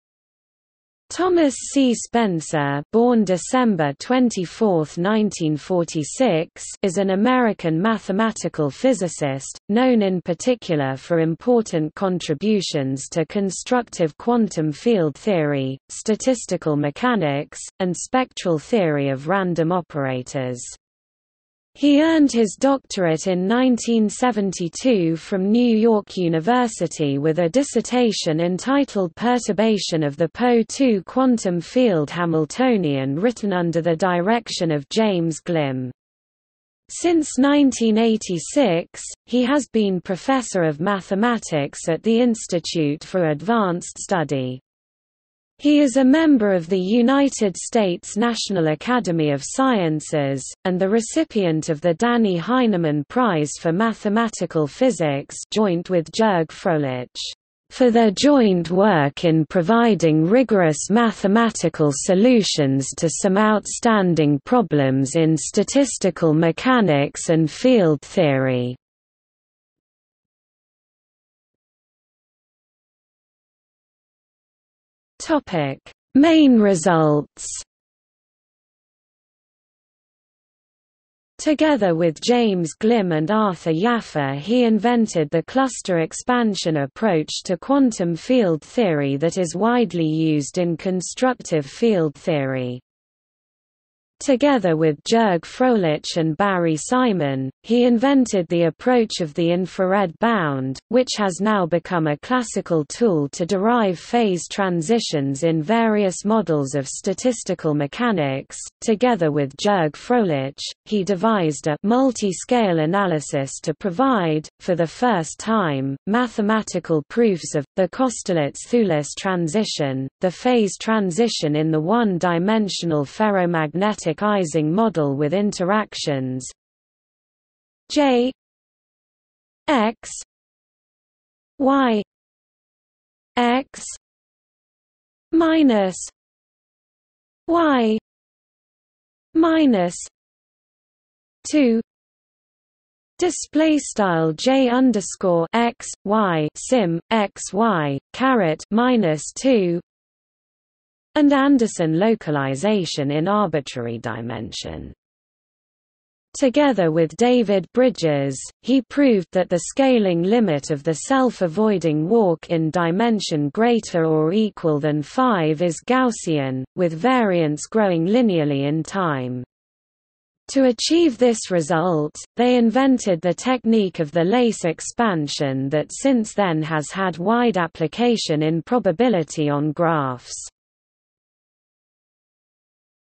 Thomas Spencer (mathematical physicist) | Wikipedia audio article
00:01:11 Main Results

- Socrates

SUMMARY
Thomas C. Spencer (born December 24, 1946) is an American mathematical physicist, known in particular for important contributions to constructive quantum field theory, statistical mechanics, and spectral theory of random operators. He earned his doctorate in 1972 from New York University with a dissertation entitled Perturbation of the Po2 Quantum Field Hamiltonian written under the direction of James Glimm. Since 1986, he has been professor of mathematics at the Institute for Advanced Study. He is a member of the United States National Academy of Sciences, and the recipient of the Dannie Heineman Prize for Mathematical Physics (joint with Jürg Fröhlich, "For their joint work in providing rigorous mathematical solutions to some outstanding problems in statistical mechanics and field theory.").